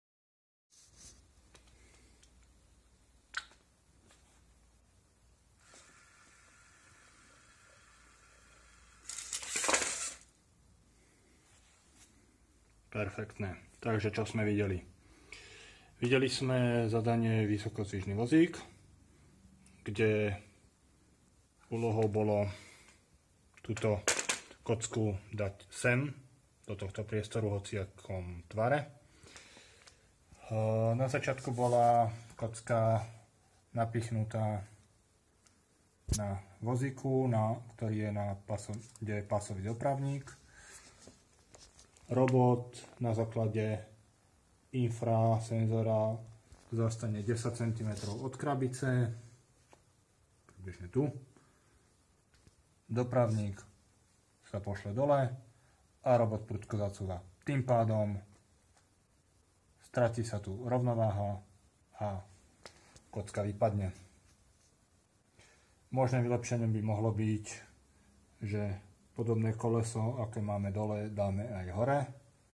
Vysokozdvižný vozík
Táto práca je súčasťou školenia pre učiteľov na LM EV3 pod vedením Mgr. Pavla Petroviča, PhD.